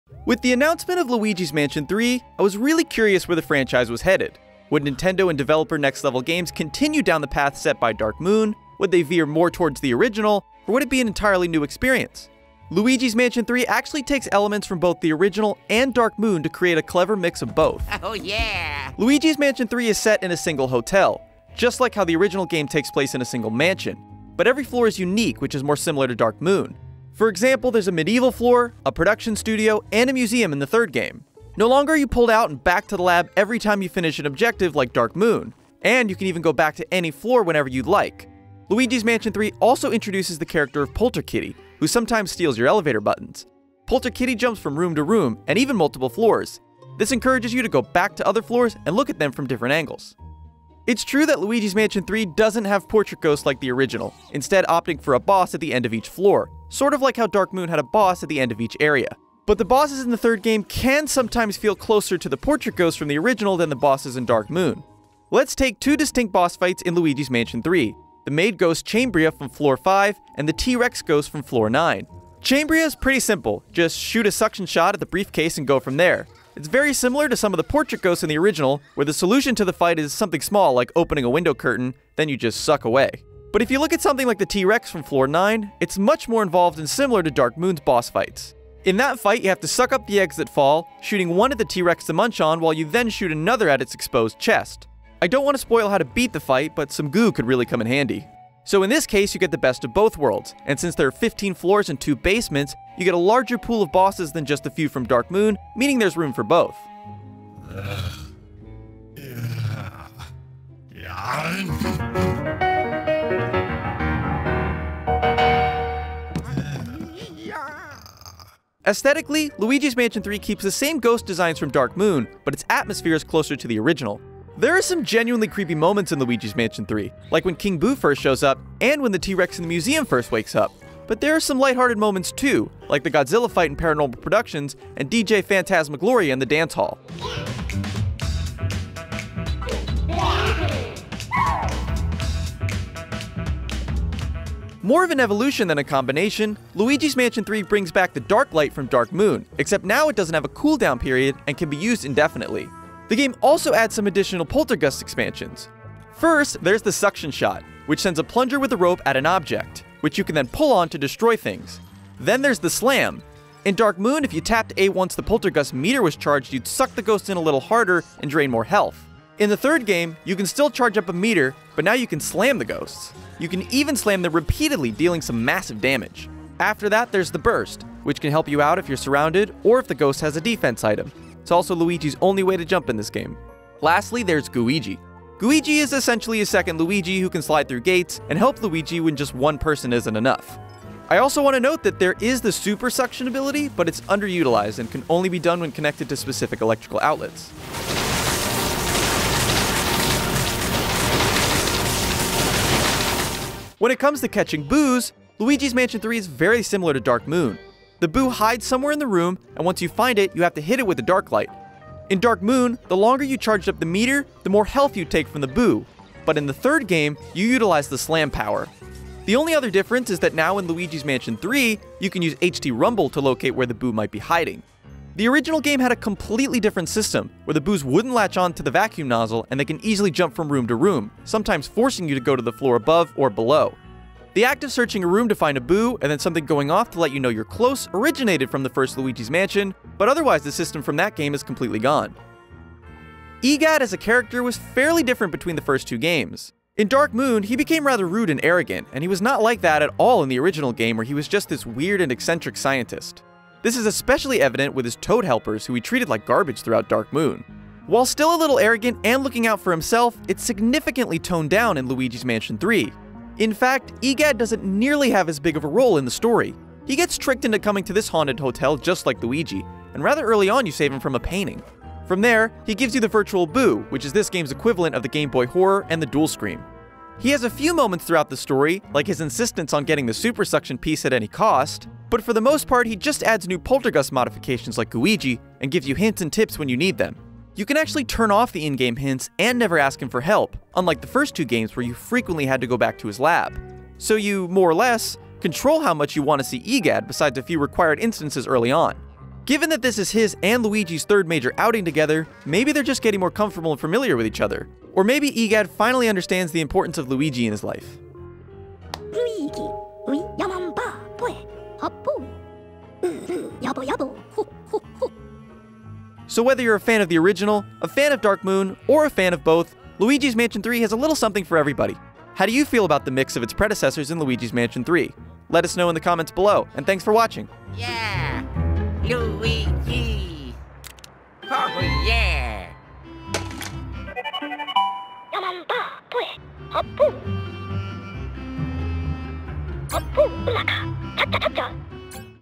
Luigi's Mansion 3 Is A Clever Mix Of The First Two Games
Did Nintendo and Next Level Games make Luigi's Mansion 3 more like Dark Moon, more like the original, or is it an entirely new experience? We take a closer look at Luigi's Mansion 3 for Nintendo Switch and analyze how its been put together.